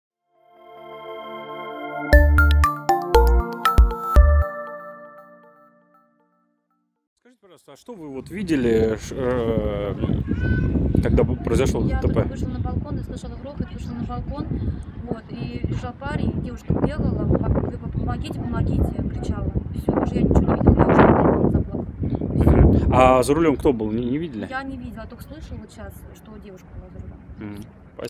В истории с ДТП в Веневе появился неизвестный молодой человек
Как передает наш корреспондент, по словам жительницы пострадавшего в Веневе дома, после ДТП она вышла на балкон и увидела автомобиль, около которого лежал парень. Возле него бегала девушка и кричала: «Помогите».

Напомним, как мы сообщали ранее, рано утром 21-летняя женщина, работающая кассиром в одном из магазинов Венёва, находясь за рулём автомобиля «Рено», не справилась с управлением и совершила съезд с автодороги. В результате автомобиль въехал под балкон расположенной на 1-м этаже квартиры 5-этажного дома в микрорайоне Южный. После дорожно-транспортного происшествия обнаружилась просадка балкона. В настоящее время принимается решение о демонтаже пяти балконов.

Сотрудники полиции выясняют обстоятельства ДТП. Водительский стаж находившейся за рулём женщины -- 1 год. Она находилась за рулём с признаками алкогольного опьянения, от медицинского освидетельствования отказалась. Пострадавших в результате ДТП нет.